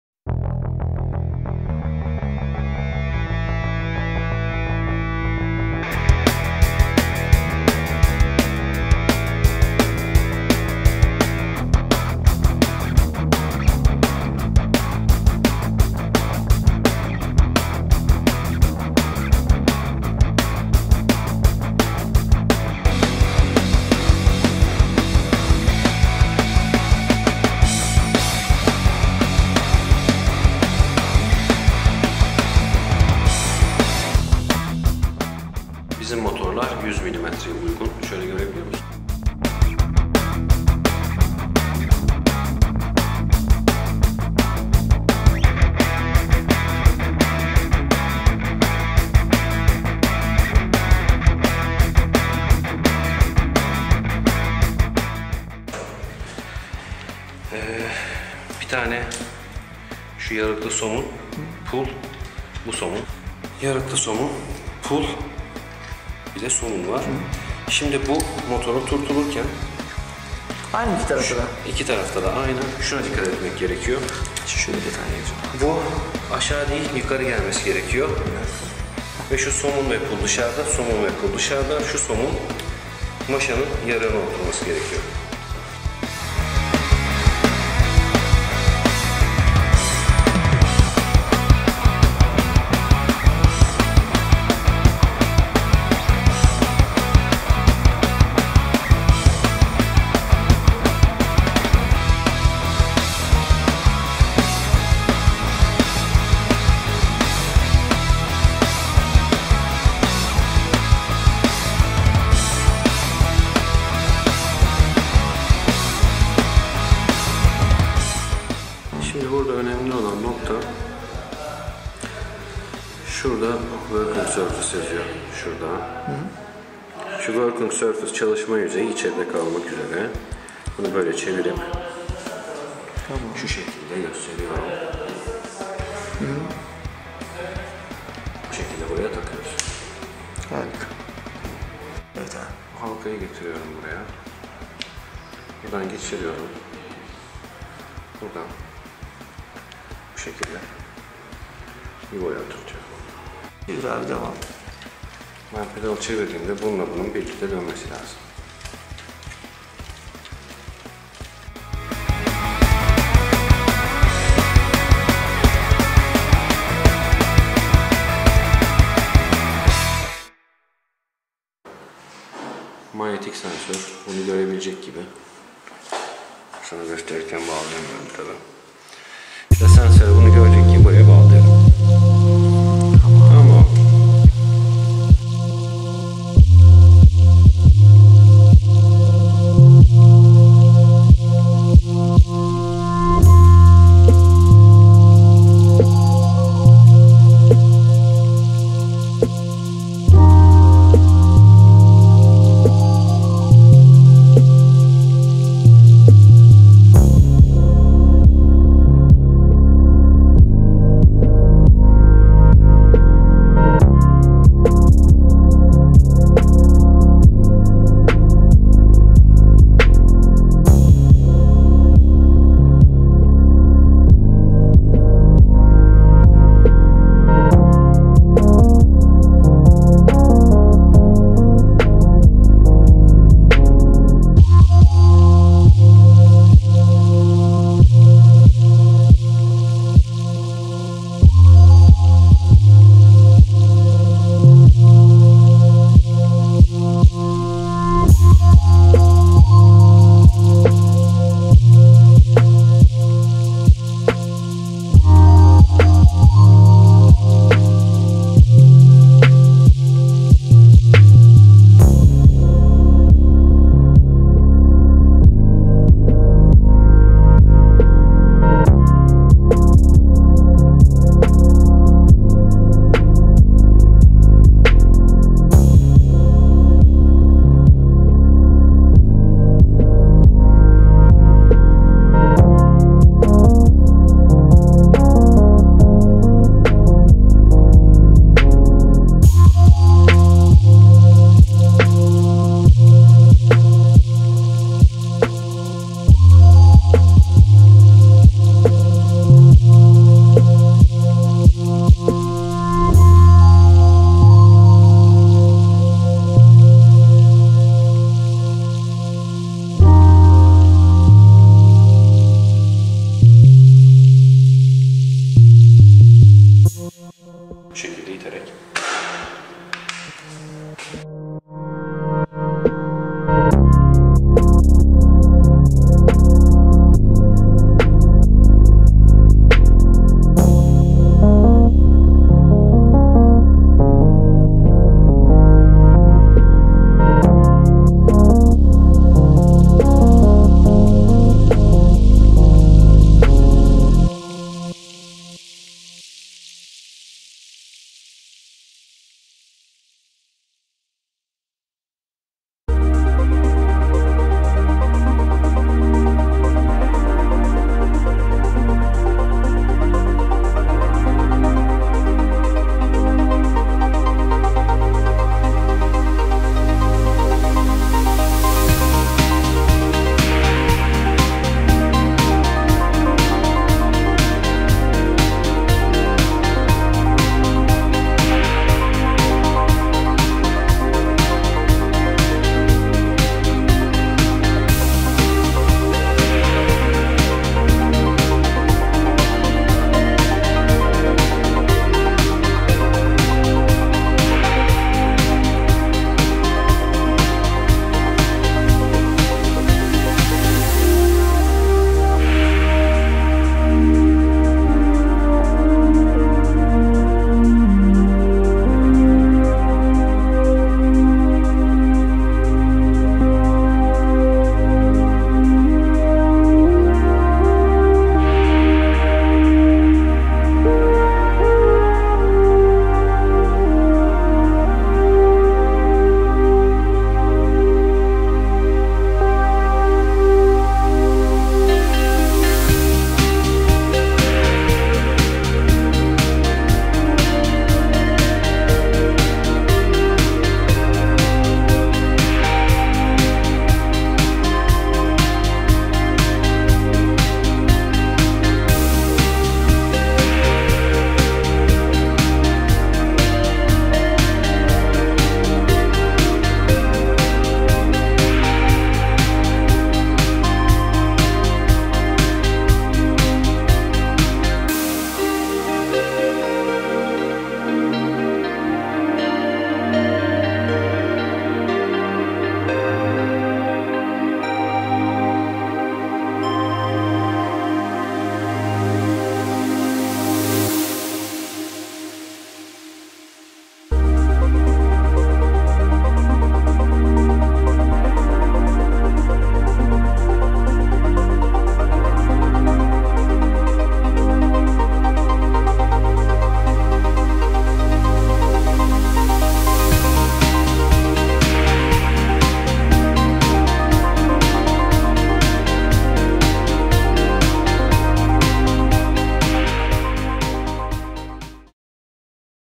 ALBA E-Bisiklet Kiti - Elektrikli Bisiklet Dönüşüm Kiti Montajı Nasıl Yapılır ?
Elektrikli Bisiklet Dönüşüm Kitlerimizin Montajı Hakkında çokça soru soruluyordu. Bu konulara açıklık getirmek üzere Çağdaş Evliye Çelebi ile birlikte bir video hazırladık.
      İyi Seyirler  !!